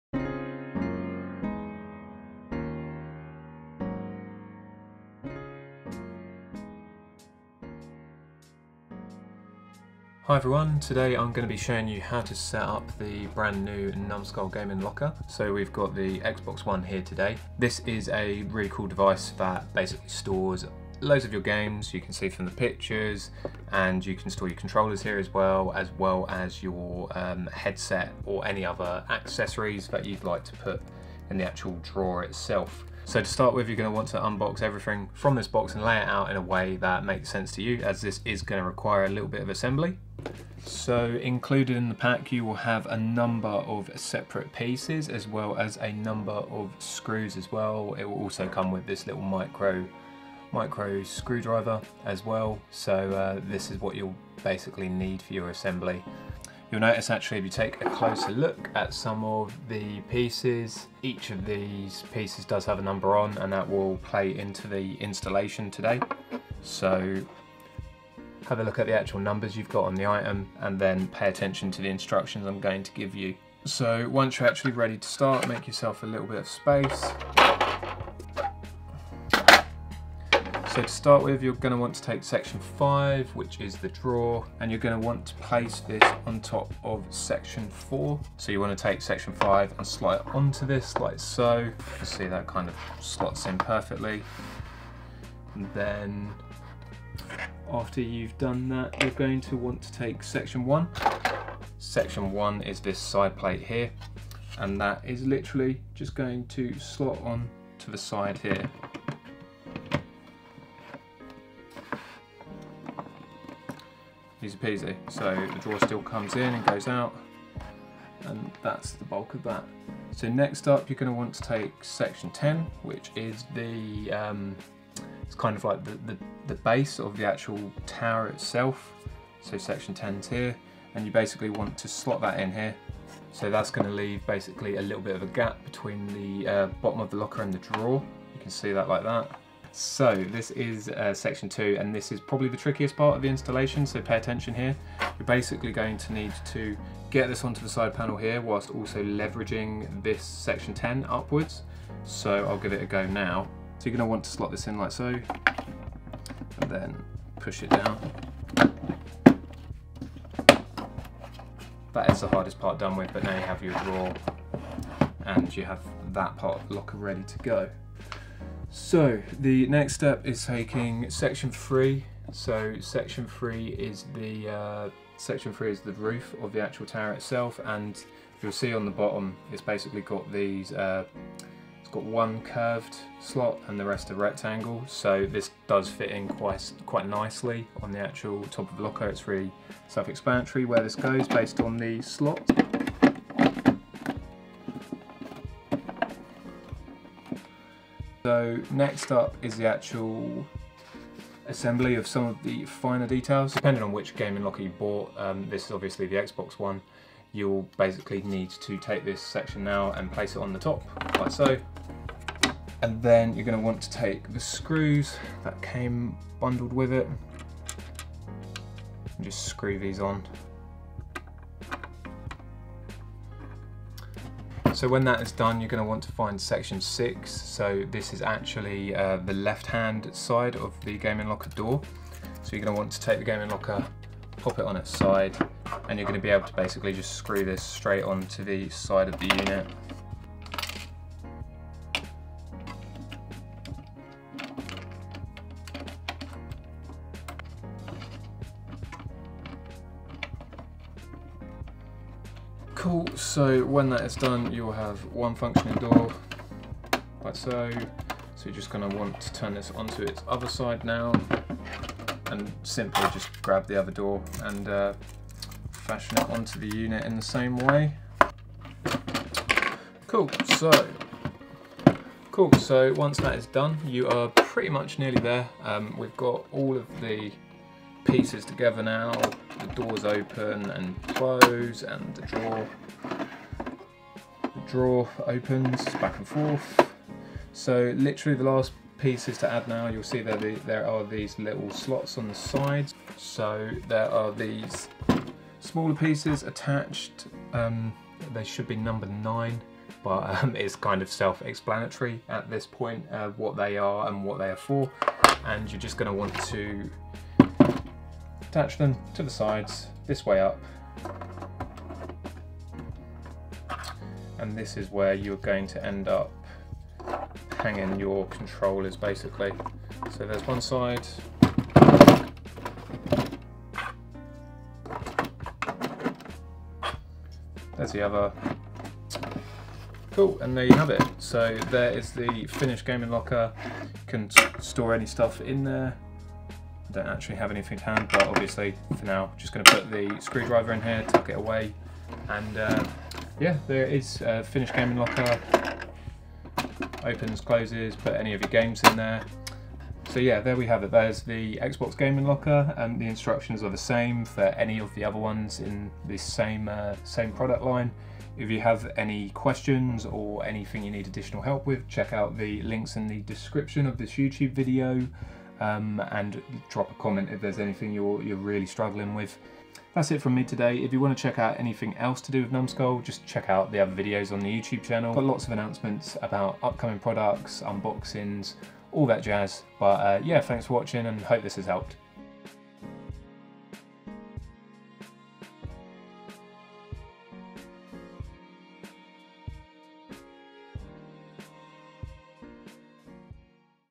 Numskull Gaming Lockers for Xbox, PS4 & Switch - Setup and Configuration
Able to hold up to 10 of your favourite games or movies, this locker features compartments to hold all your essentials such as controllers, cables and more. Not to mention it’ll securely mount your headphones on the top. This is the perfect gaming accessory to keep all your games and accessories organised.

If you need some assistance building the Numskull Gaming Locker this video should hopefully get you on track w/ a step by step guide.